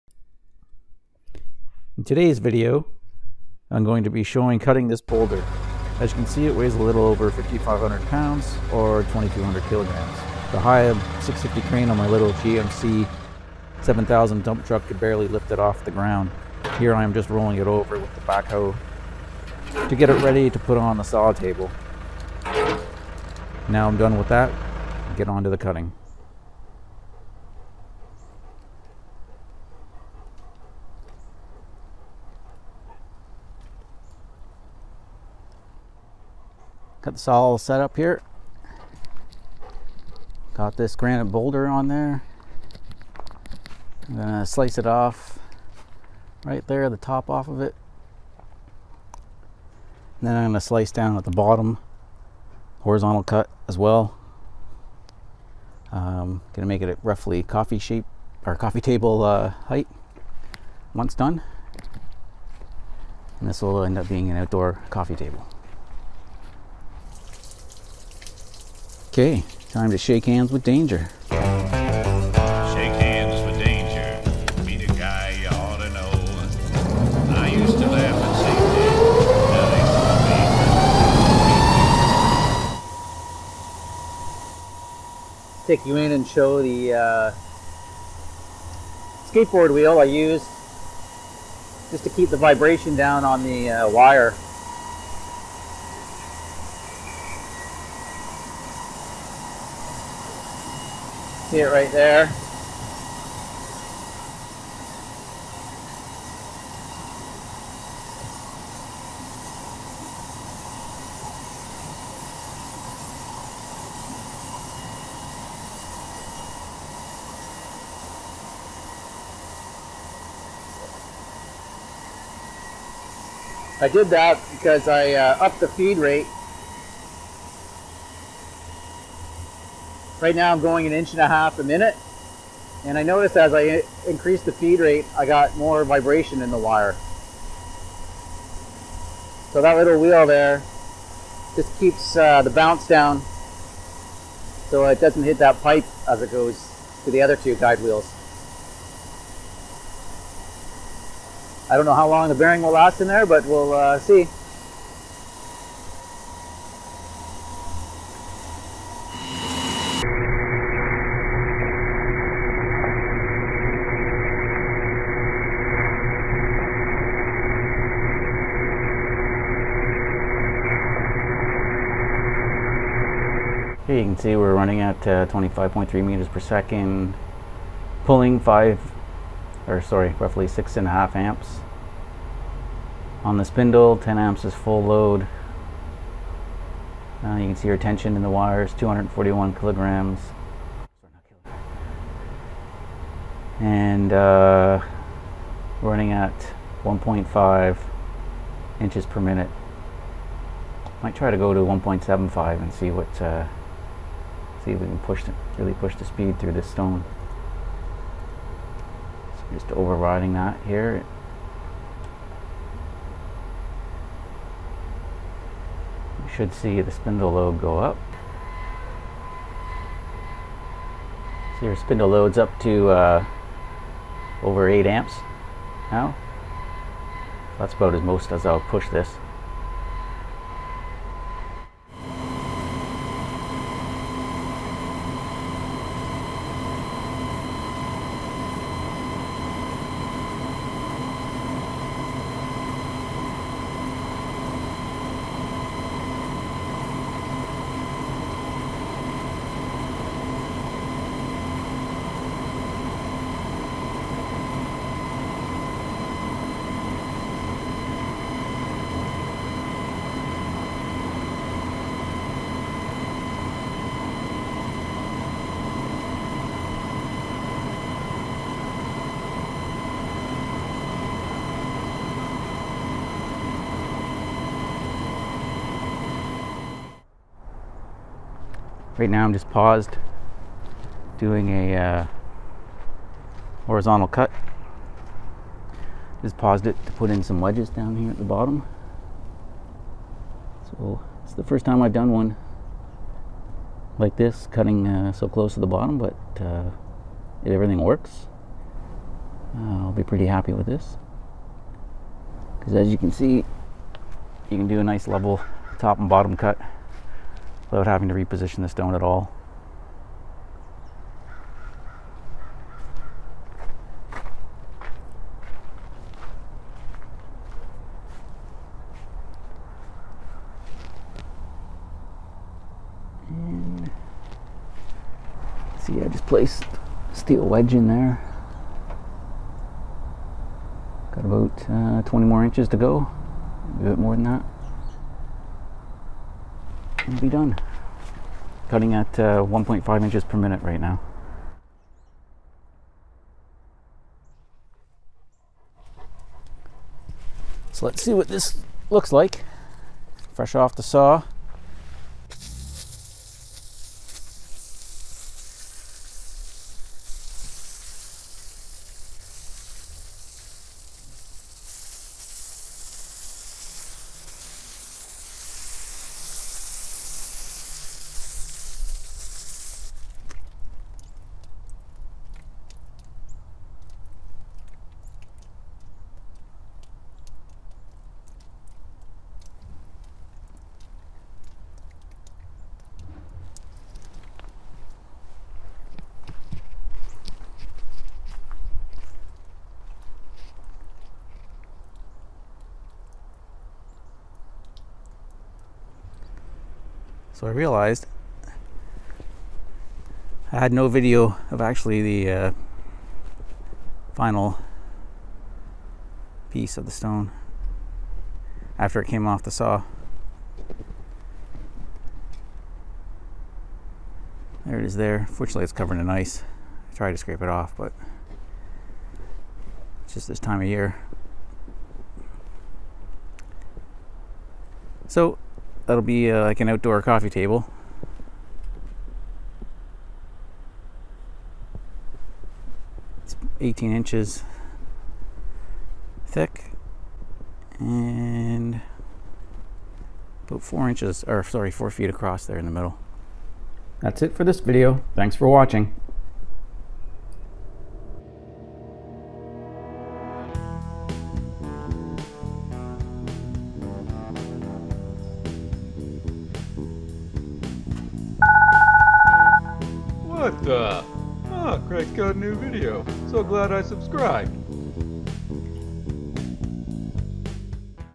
Cutting Granite Boulder with diamond wire saw.
I show the process of cutting a 5500 lb granite boulder with a diamond wire saw.  In this case the stone was cut to make a simple outdoor coffee table.
Diamond wire purchased through - Husqvarna construction